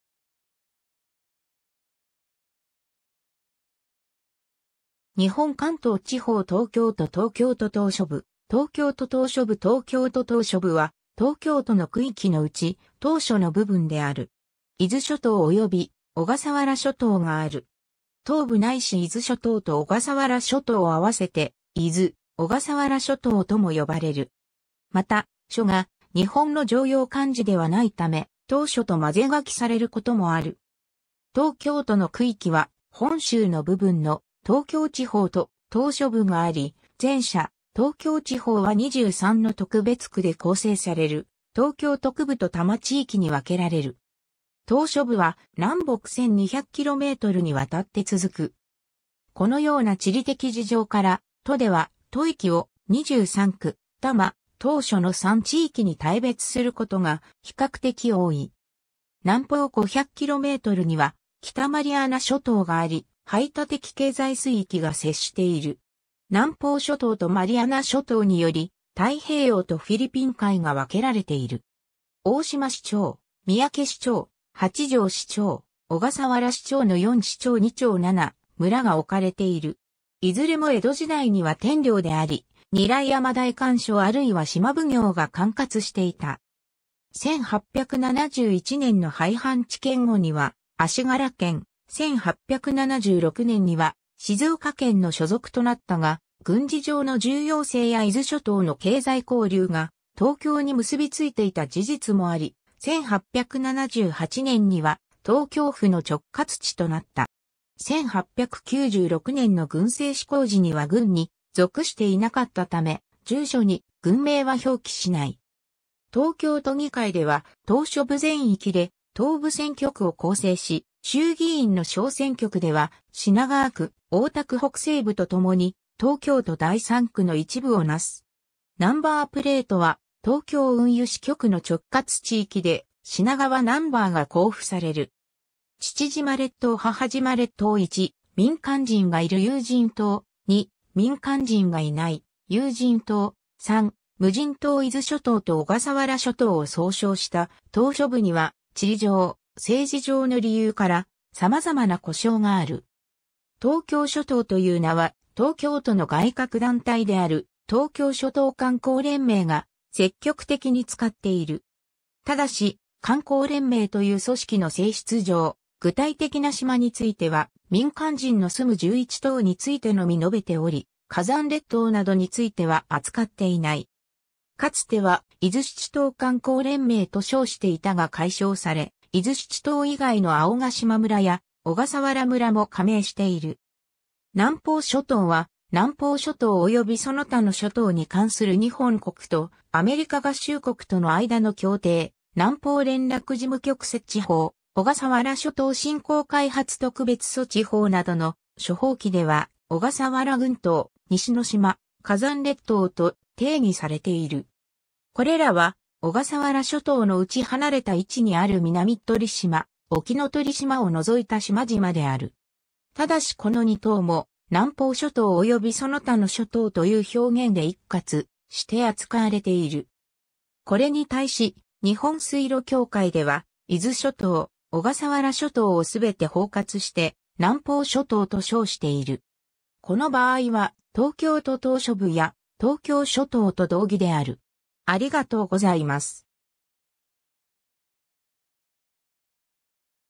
東京都島嶼部
日本 」 関東地方 」 東京都 」 東京都島嶼部 東京都島嶼部（黒字は小笠原諸島） 東京都島嶼部（とうきょうととうしょぶ）は、東京都の区域の内、島嶼の部分である。
伊豆諸島及び小笠原諸島がある。
 「島部」ないし伊豆諸島と小笠原諸島を併せて「伊豆・小笠原諸島」とも呼ばれる。
また「嶼」が日本の常用漢字ではないため、「島しょ」と交ぜ書きされることもある（東京都島しょ農林水産総合センター、東京島しょ農業協同組合など）。
 東京都の区域は本州の部分（東京港内の島を含む）の「東京地方」と島嶼部があり、前者「東京地方」は23の特別区で構成される東京都区部（23区）と多摩地域（市郡）に分けられる。
島嶼部は南北1200kmにわたって続く。
このような地理的事情から、都では都域を「23区・多摩・島嶼」の三地域に大別することが比較的多い。
 南方500kmには北マリアナ諸島があり、排他的経済水域が接している。
南方諸島（狭義）とマリアナ諸島により、太平洋とフィリピン海が分けられている。
 大島支庁（大島町・利島村・新島村・神津島村）・三宅支庁（三宅村・御蔵島村）・八丈支庁（八丈町・青ヶ島村・ほか直轄地）・小笠原支庁（小笠原村）の4支庁2町7村が置かれている。
いずれも江戸時代には天領であり、韮山代官所あるいは島奉行が管轄していた。
1871年の廃藩置県後には足柄県、1876年には静岡県の所属となったが、軍事上の重要性や伊豆諸島の経済交流が東京に結びついていた事実もあり、1878年には東京府の直轄地となった。
1896年の郡制施行時には郡に属していなかったため、住所に郡名は表記しない。
 東京都議会では島嶼部全域で島部選挙区（定数1）を構成し、衆議院の小選挙区では品川区・大田区北西部と共に東京都第3区の一部を為す。
ナンバープレートは東京運輸支局の直轄地域で「品川」ナンバーが交付される。
 父島列島 母島列島 ※1：民間人がいる有人島、 ※2：民間人がいない有人島、 ※3：無人島 伊豆諸島と小笠原諸島を総称した島嶼部には地理上・政治上の理由から様々な呼称がある。
 東京諸島（とうきょうしょとう）という名は、東京都の外郭団体である「東京諸島観光連盟」が積極的に使っている。
ただし観光連盟という組織の性質上、具体的な島については民間人の住む11島についてのみ述べており、火山列島などについては扱っていない。
 かつては「伊豆七島観光連盟」と称していたが改称され、伊豆七島以外の青ヶ島村や小笠原村も加盟している。
 南方諸島（なんぽうしょとう）は「南方諸島及びその他の諸島に関する日本国とアメリカ合衆国との間の協定」（小笠原返還協定）、「南方連絡事務局設置法」、「小笠原諸島振興開発特別措置法」などの諸法規では小笠原群島・西之島・火山列島と定義されている。
これらは、小笠原諸島のうち離れた位置にある南鳥島・沖ノ鳥島を除いた島々である。
ただしこの2島も、「南方諸島及びその他の諸島」という表現で一括して扱われている。
 これに対し、日本水路協会では伊豆諸島・小笠原諸島を全て包括して「南方諸島」と称している。
この場合は「東京都島嶼部」や「東京諸島」と同義である。